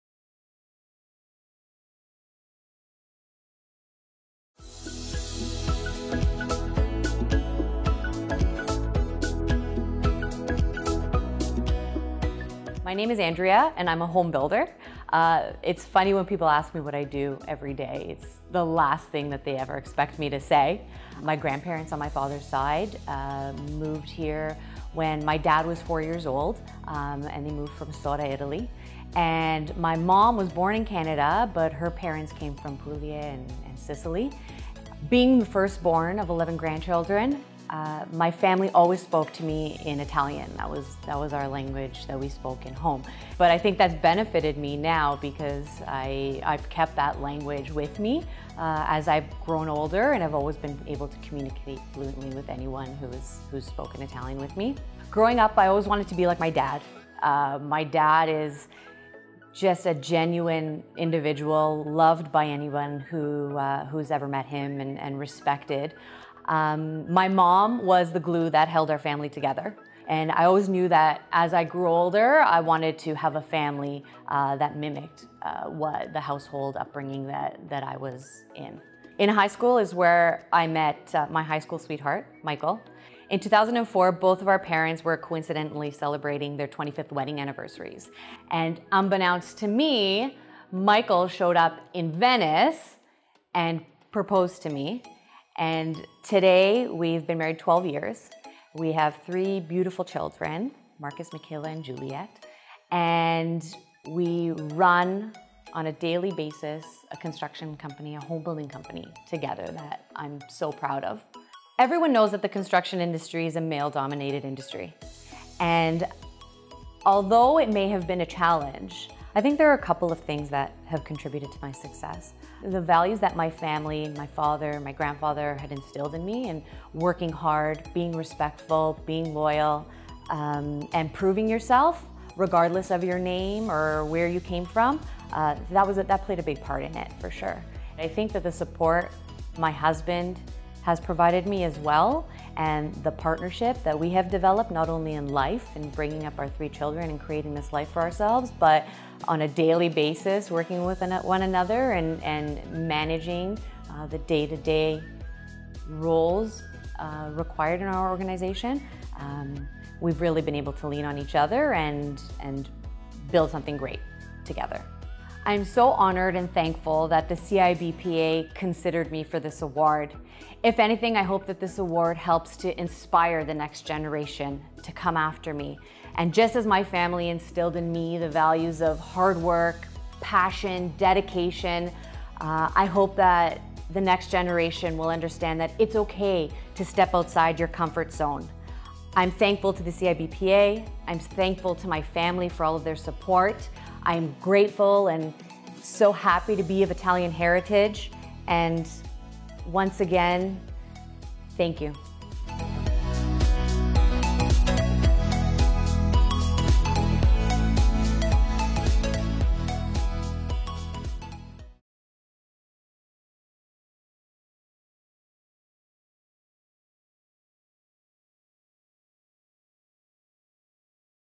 Andrea DeGasperis-Ronco Receives Next Generation Award | 66th Annual CIBPA President's Ball
Learn more about Andrea's accomplishments and achievements throughout her life and career as she accepts this year's Next Generation Award from the Canadian Italian Business Professional Association's 66th Annual President's Ball.